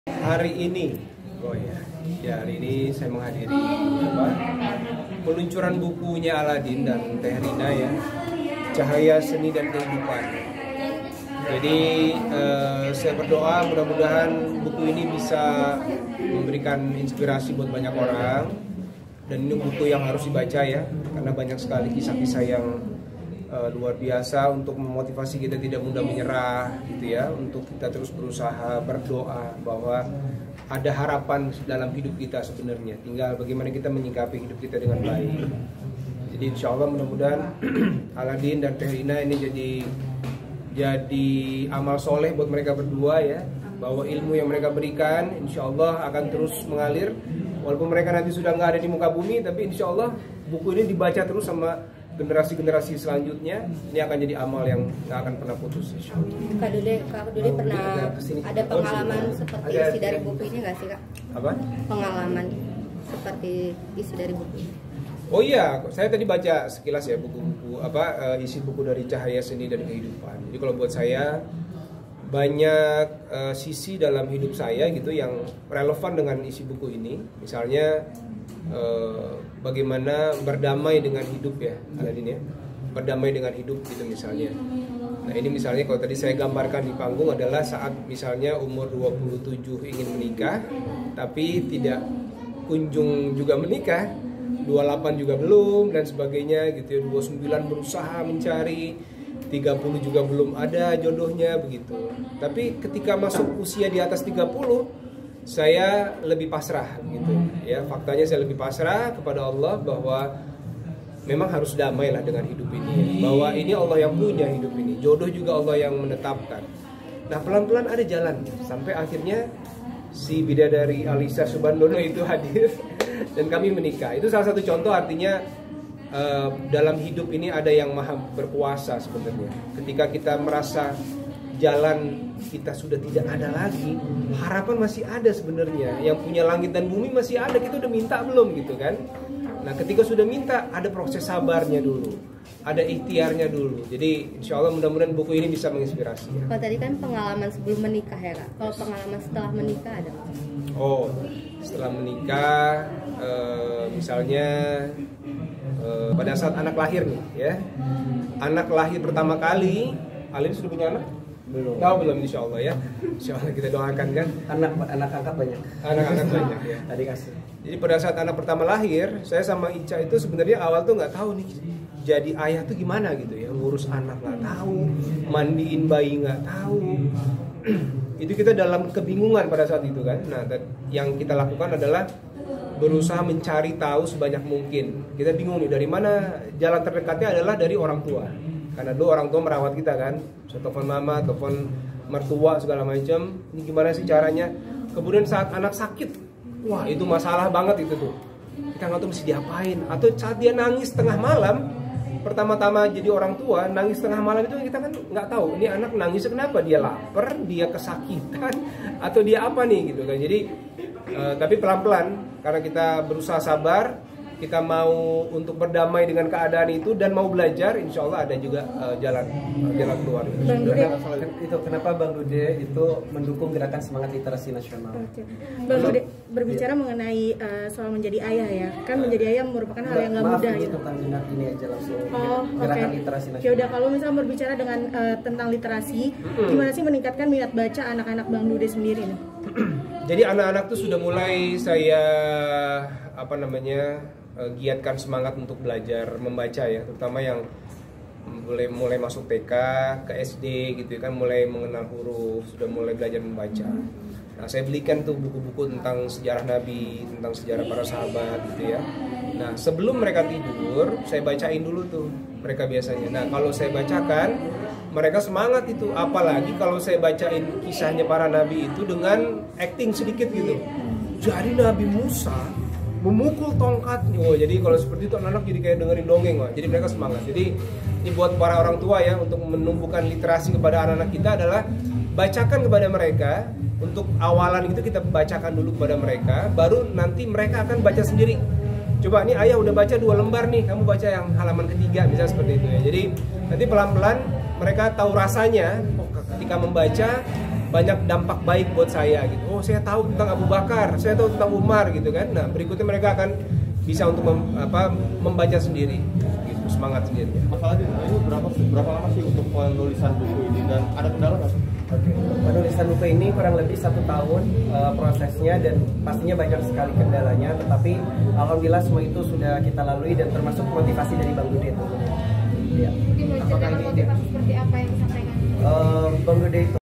Dude Harlino Blak-blakan Bongkar Kisah Cintanya Dengan Alyssa Subandono
WARTAKOTALIVE.COM, JAKARTA -
Nama Dude Harlino tidak asing lagi di dunia sinetron Indonesia.

Dude Harlino baru saja menceritakan pengalaman hidupnya yang sulit mendapatkan jodoh, sampai akhirnya menikah dengan Alyssa Soebandono.

Dude dan Alyssa resmi menikah pada 2014 dengan perbedaan usia sebanyak 11 tahun lamanya.

Pengalamannya ini diungkapkannya, karena adanya kesamaan dalam cerita di buku berjudul Cahaya, Seni, dan Kehidupan.